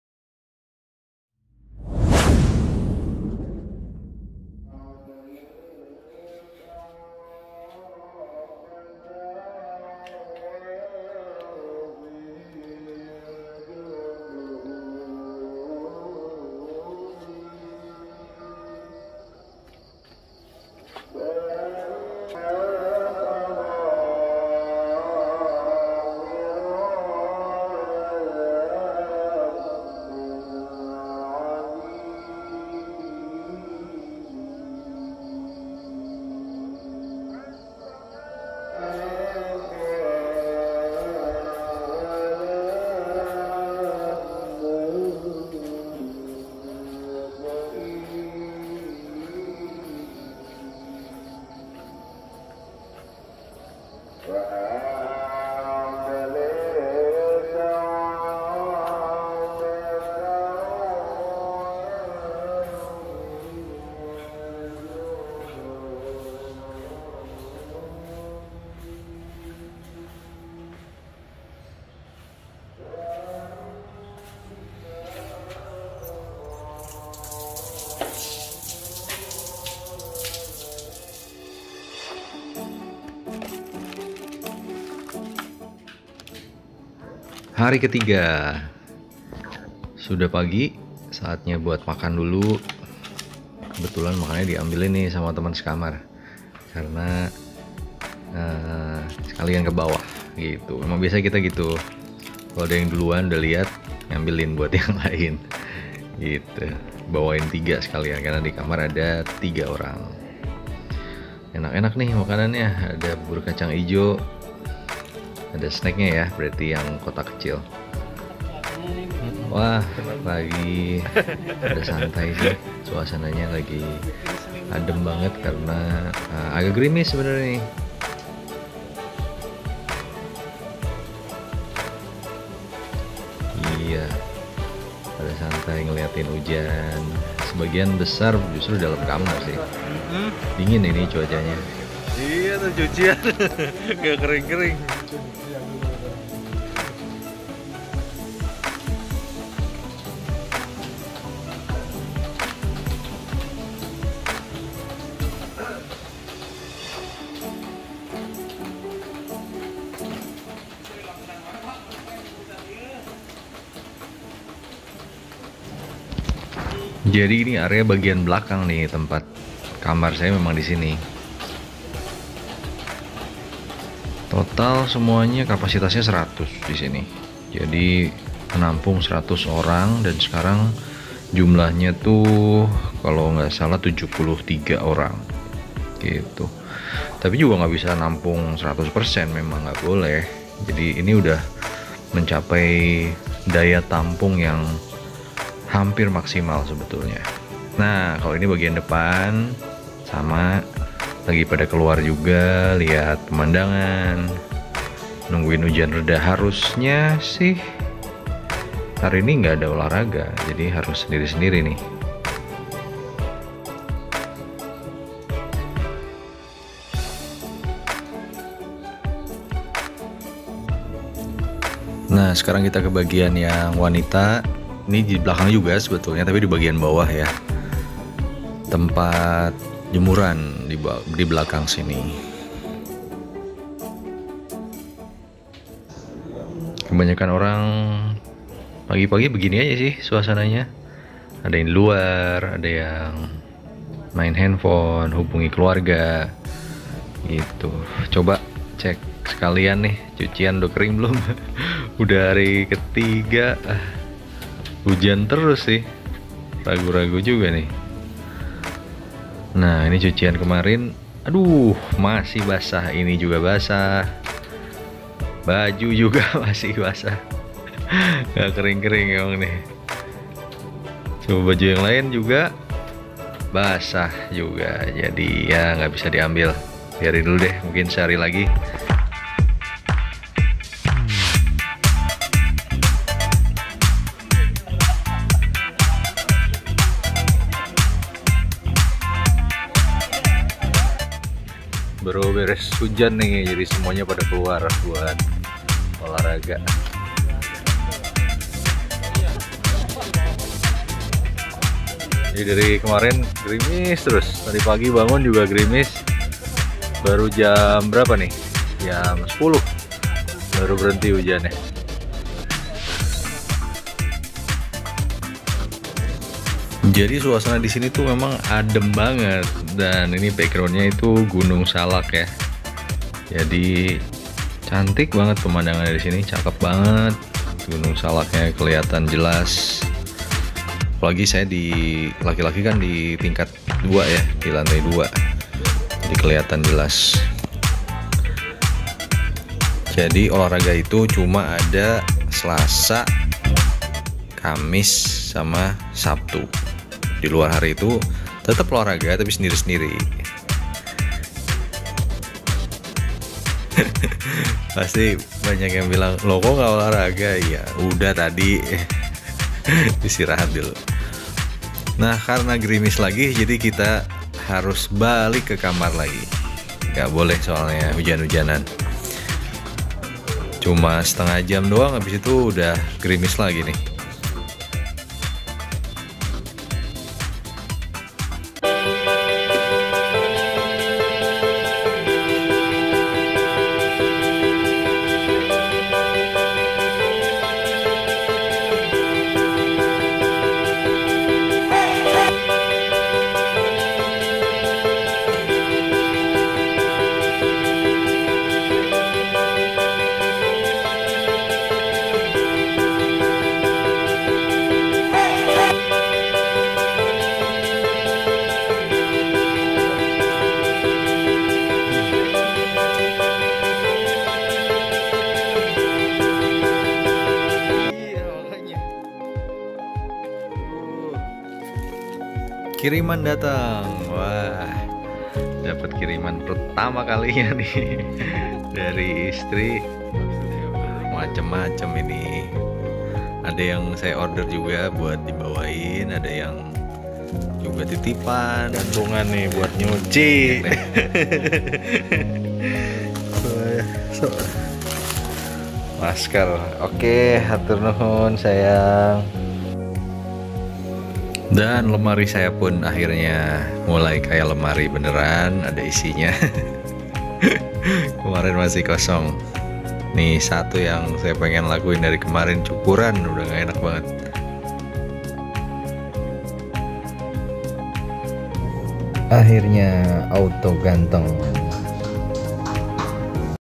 DAY 3 Positif Covid-19 - ISOLASI LIDO - INDONESIA - PENGALAMAN ISOLASI COVID-19
Semangat dan tetap berpikir positif. Kata-kata ini yang paling sering saya terima dari ratusan pesan dan komen yang masuk. Alhamdulillah.

Memang itu yang diperlukan oleh orang-orang di sini. Terpisah dari keluarga dan tidak bisa bekerja, pasti jadi alasan untuk segera keluar dari situasi tidak diharapkan ini.

Kasih semangatt yaa...